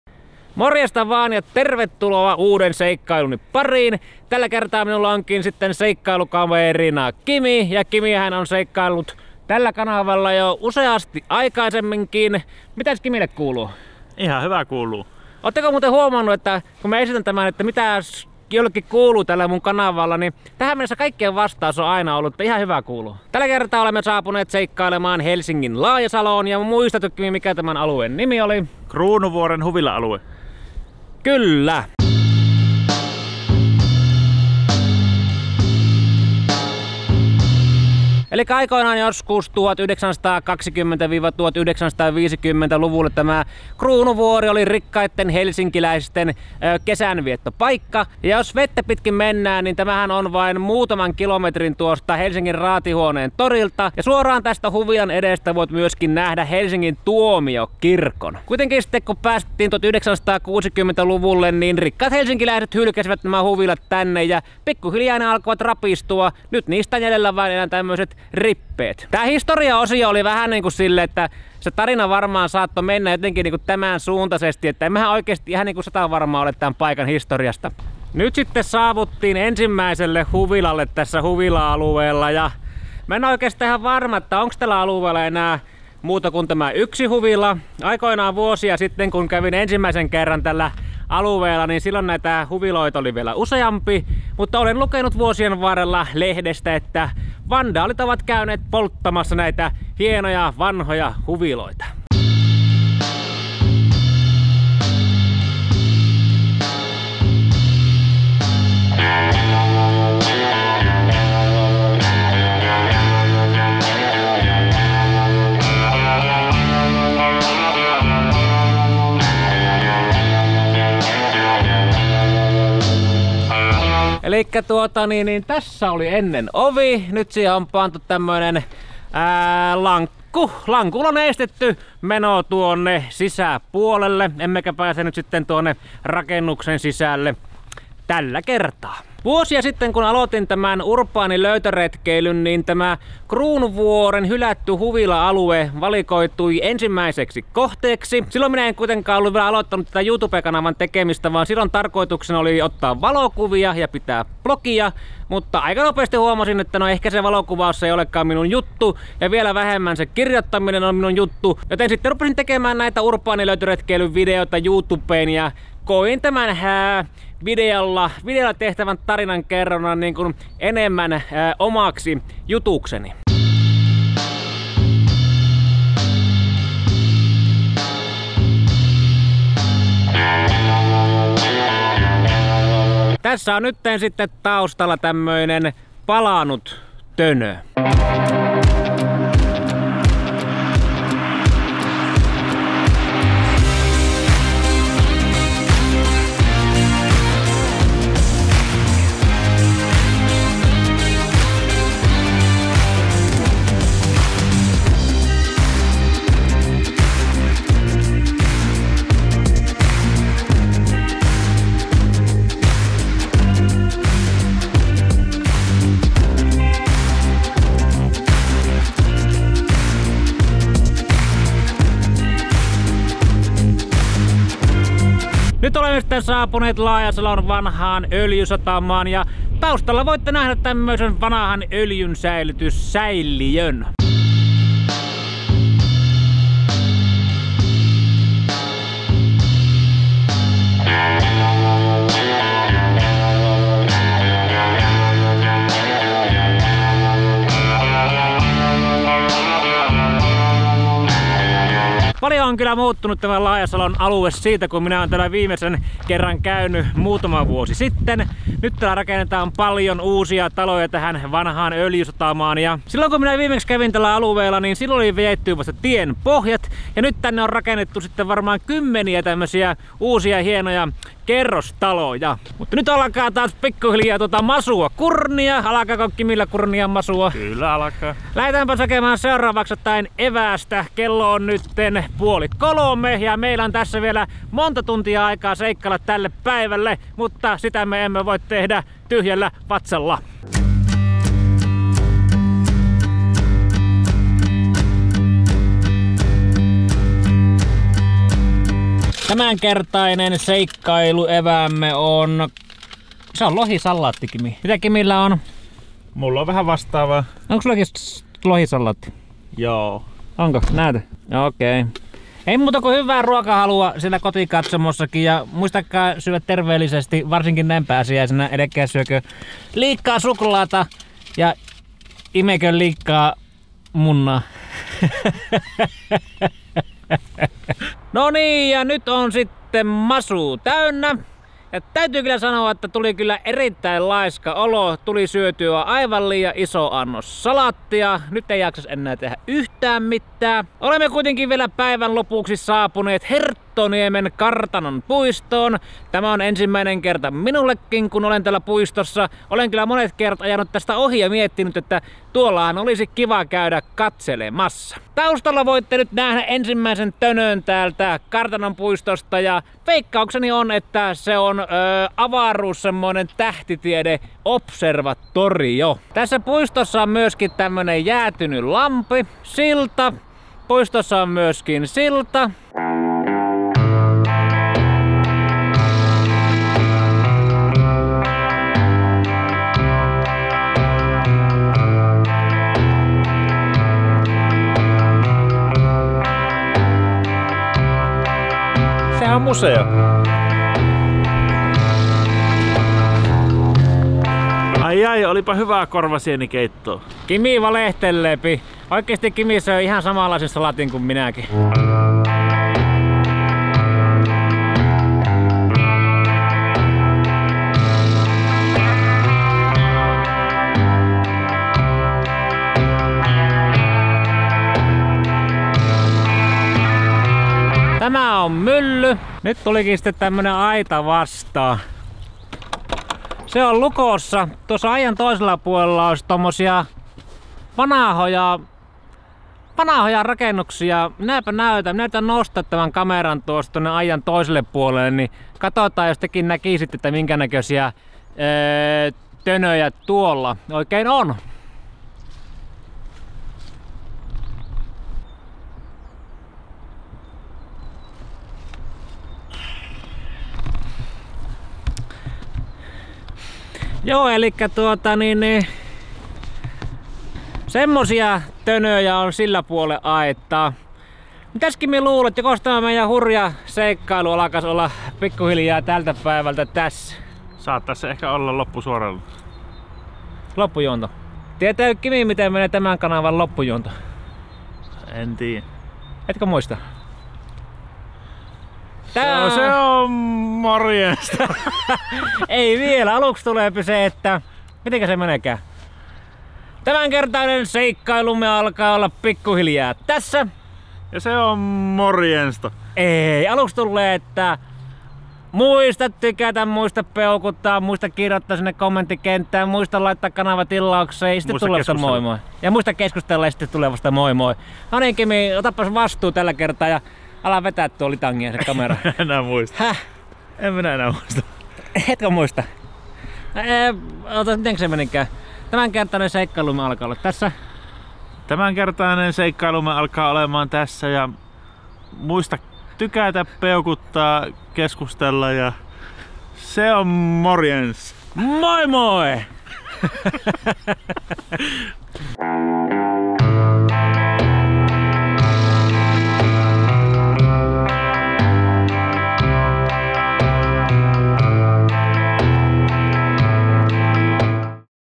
Kruunuvuoren viimeinen huvila - huvila paloi maan tasalle | viimeistä kertaa videolla HELSINKI
Kävimme seikkailemassa Helsingin Laajasalossa pääsiäisenä 2021. Seikkailumme alkaa Kruunuvuoren huviloista. Siitä jatkoimme matkaa Laajasalon vanhaan öljysatamaan. Lopuksi kävimme vierailulla vielä Herttoniemen kartanossa.

Kruunuvuoren huvila tuhopoltettiin pari viikkoa kuvausten jälkeen. Tämä video saattaa olla viimeinen video, jolla Kruunuvuoren viimeinen huvila esiintyy viimeistä kertaa.

Tämä video on läpileikkaus hylättyyn Laajasaloon. Tällä alueella on mielenkiintoinen historia ja unohtumattomia kohteita. Sivistys on löytänyt Laajasalon ja alue muuttuu kovaa vauhtia. Sinne on rakennettu valtavasti uusia asuntoja ja hylätyistä kohteista on tehty nähtävyyksiä.

Käy katsomassa video ja nauti matkasta. Muista laittaa kanava tilaukseen niin pääset mukaan aina uudestaan ihmeellisille seikkailuille..